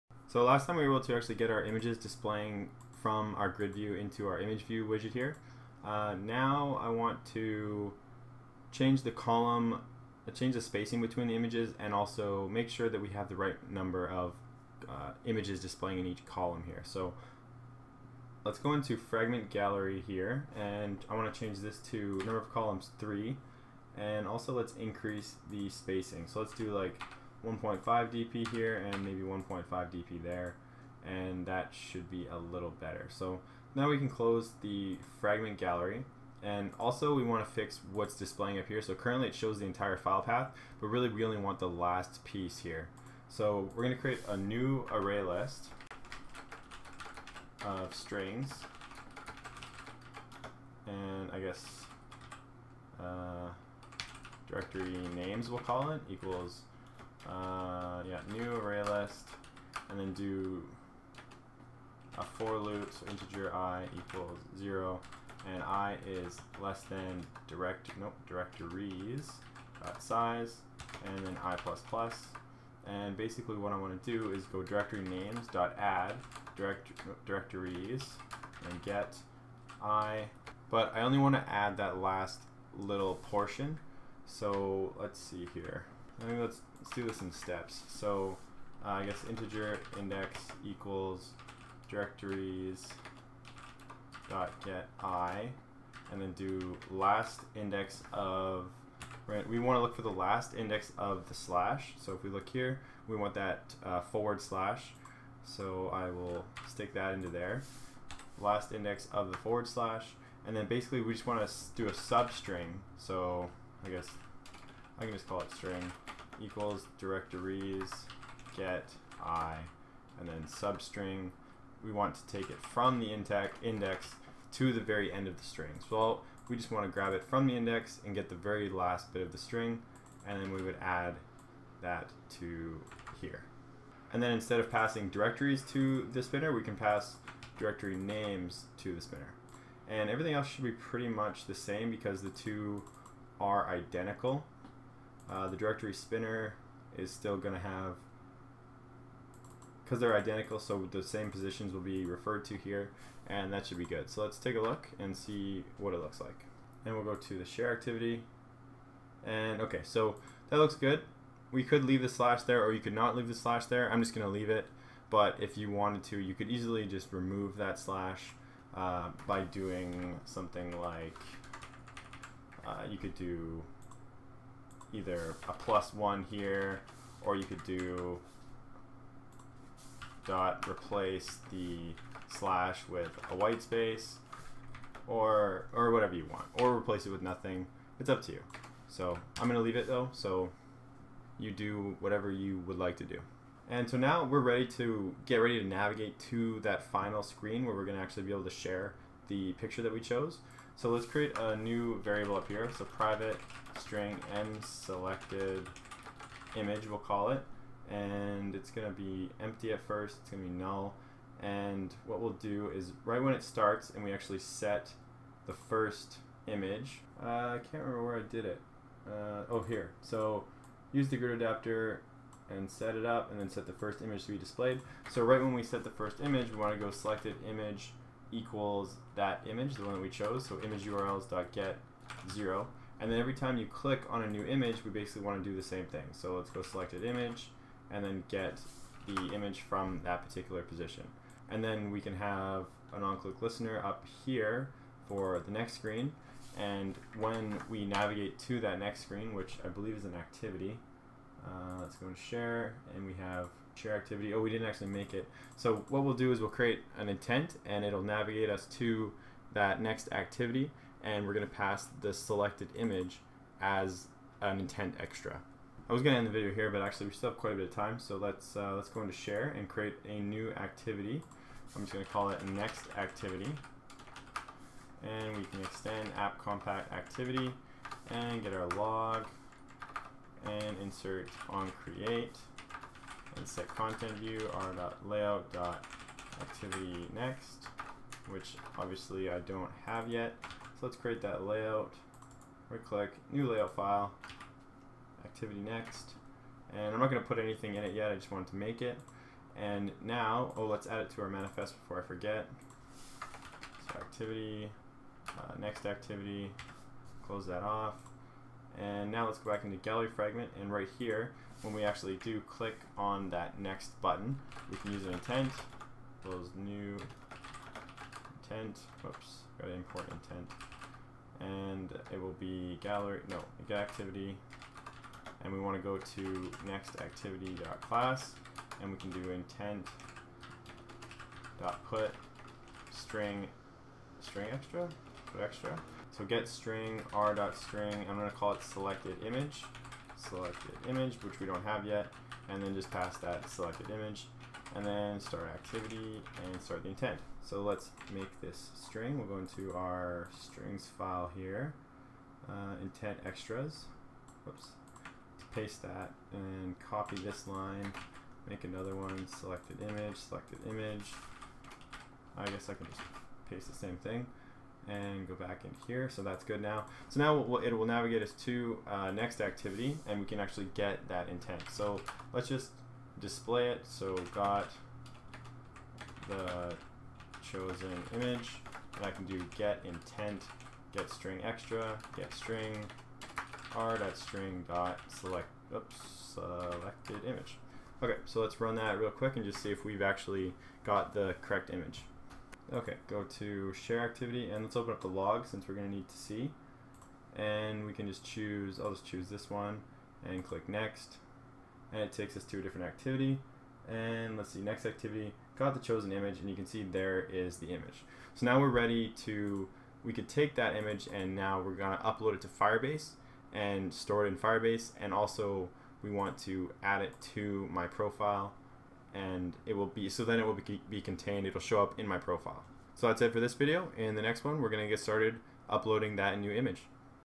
Selected Image to Share (Part 48) - [Build an Instagram Clone]
In this video we choose an image for sharing to the Instagram clone.